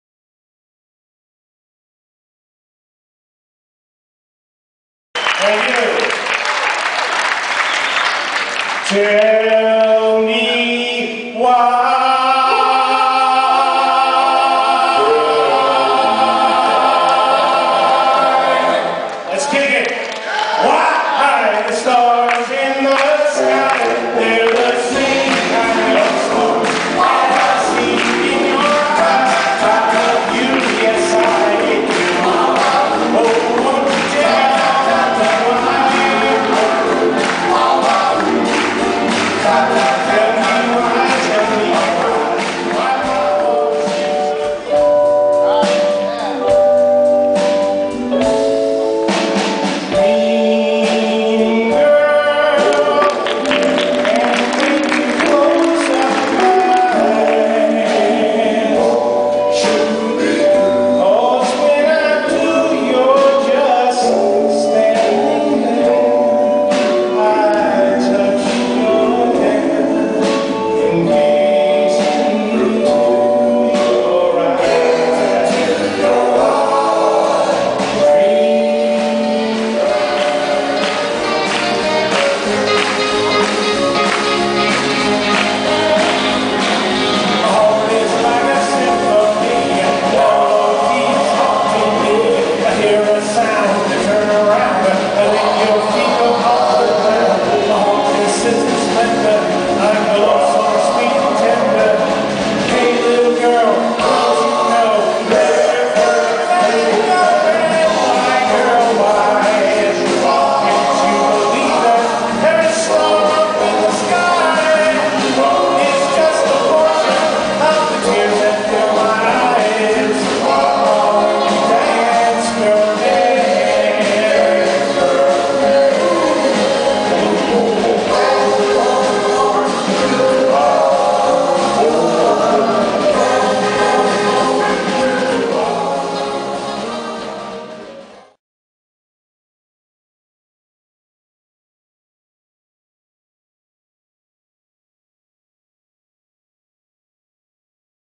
Norman Fox and the Rob Roys at the ULTIMATE DOO-WOP EVENT...DOO-WOP WEEKEND!!!
A Live compilation at the great annual Doowop event in Nov.